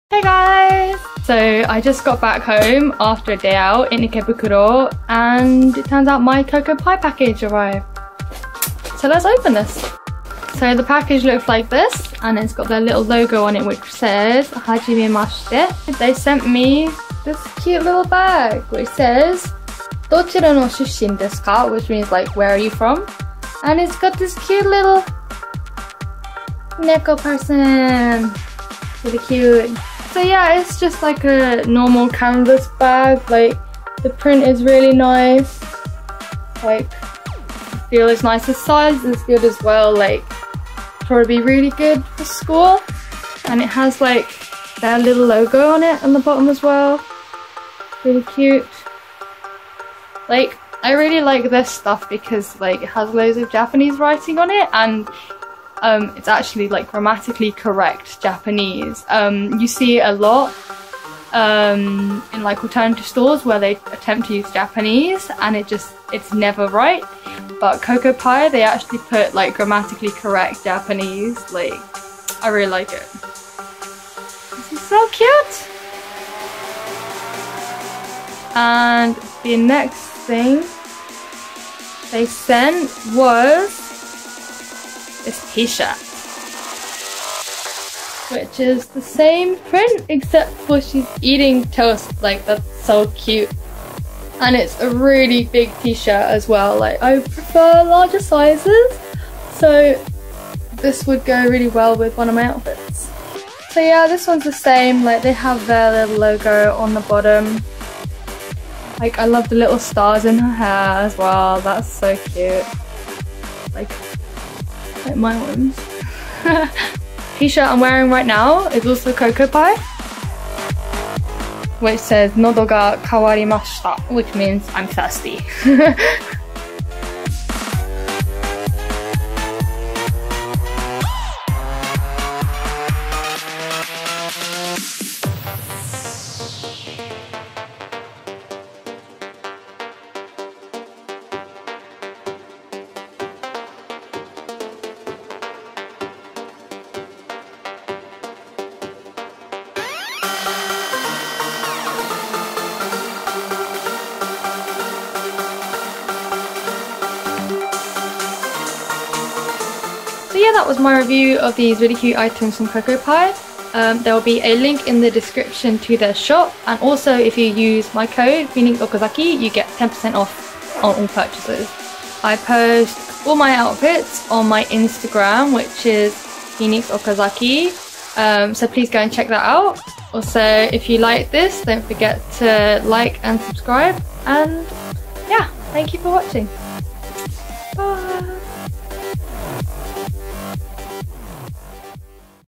☆ ~ KOKOpie Review! ~ ☆
Hey guysss ~ !

So the brand I model for, KOKOpie, send me some cute stuff! :D so I thought I'd film a review ~ I didn't know what they were sending me, so I was super happy when it turned out to be their cat girl prints! ahhh XDD